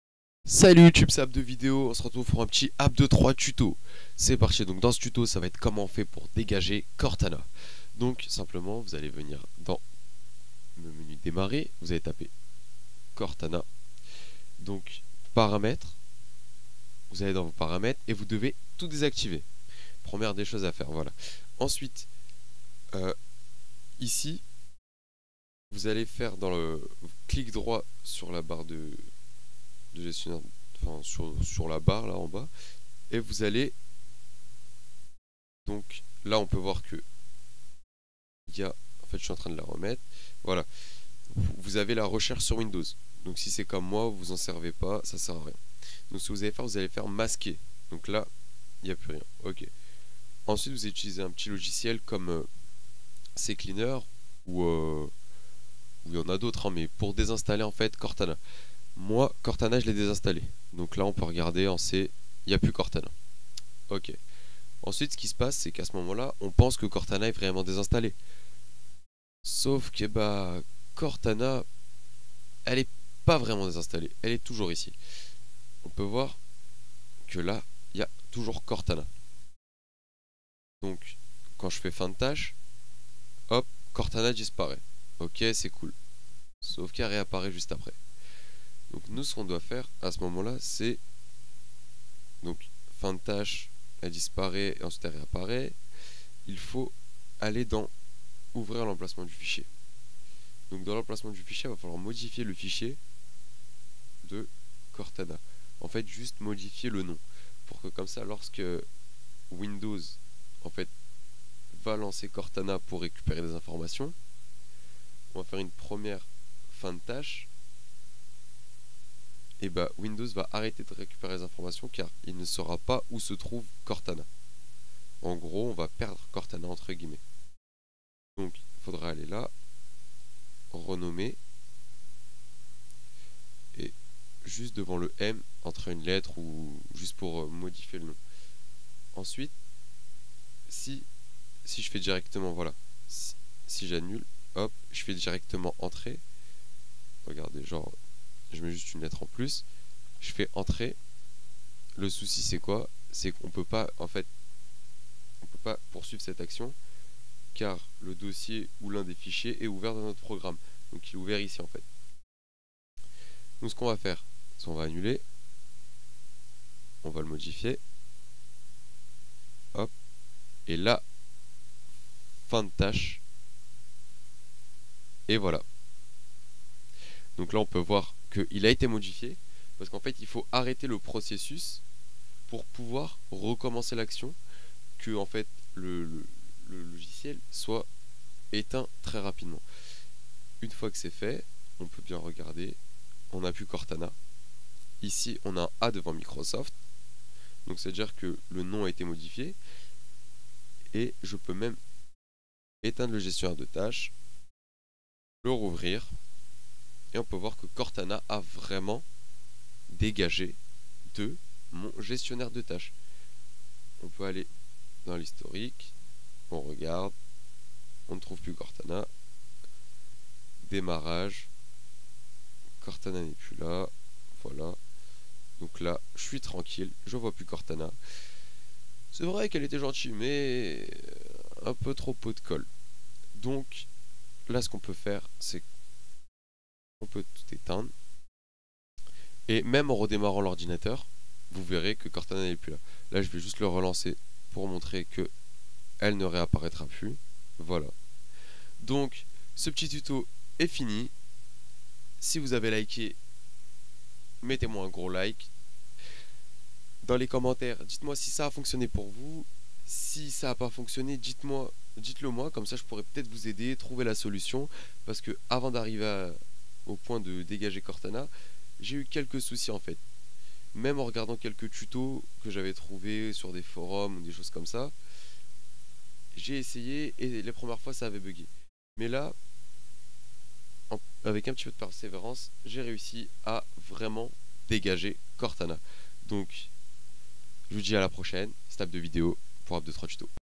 COMMENT DÉSINSTALLER CORTANA de WINDOWS 10 ?
Salut c'est AbdeVideo nouveau petit tuto pour enlever cortana de windows 10.
pour réutilisé cortana modifié le mon du fichier comme a la base et redémarré la session .